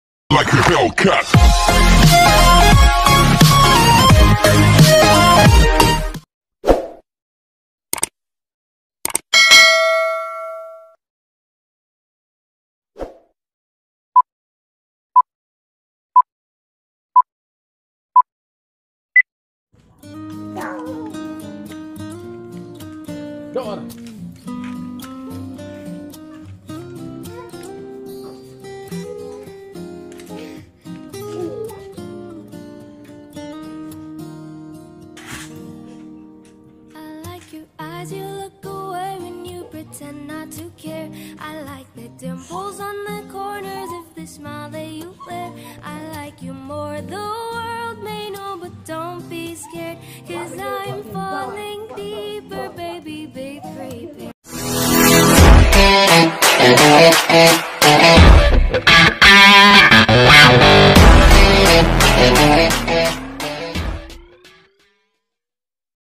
anak kecil memang lucu ya [ lagi lucu" nya dan dan suara tembakan nya 😁😁]
instagram egysta saputra